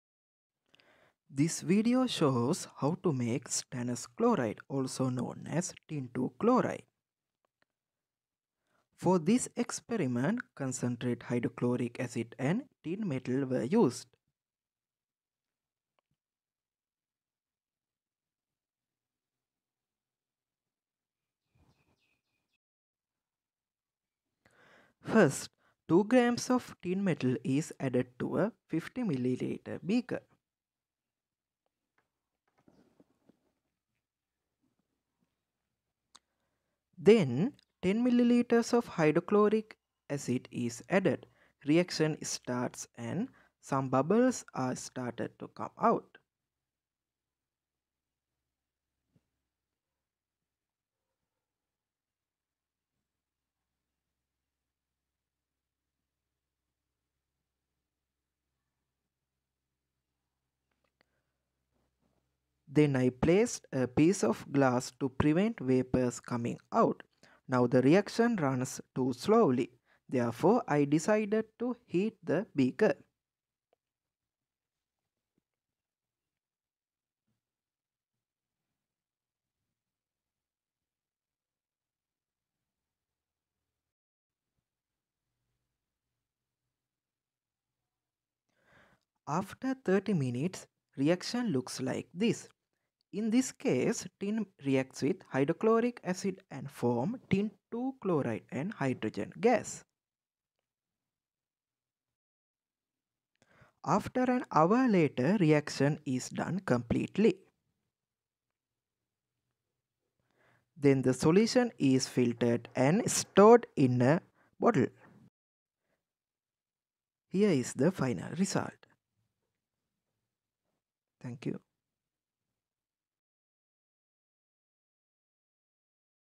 How to make stannous chloride ( Tin chloride )
Stannous Chloride ; Also known as Tin (ll) chloride. High hygroscopic  compound. Tin(II) chloride is a white crystalline solid. It forms a stable dihydrate, but aqueous solutions tend to undergo hydrolysis, particularly if hot. SnCl₂ is widely used as a reducing agent, and in electrolytic baths for tin-plating.

Warning !

Hydrochloric acid is corrosive. Must wear safety equipment when handling  hydrochloric acid.( lab coat, gloves, safety glasses).
This video shows how to make stannous chloride from tin metal. Tin chloride making process is very simple one, by reacting tin metal with hydrochloric acid. Because of slow reactivity of tin metal, this reaction is happened very slowly. Therefore the mixture is heated with bunsen burner or spirit lamp.

Sn(s)  +  2HCl(conc)     =   SnCl2(aq)   + H2(g)
In my case it took 60 minutes to complete the reaction.

Coming up videos..
Tin dendrite - complete guide
Nickel DMG preparation
Identify gold, platinum, palladium by using tin chloride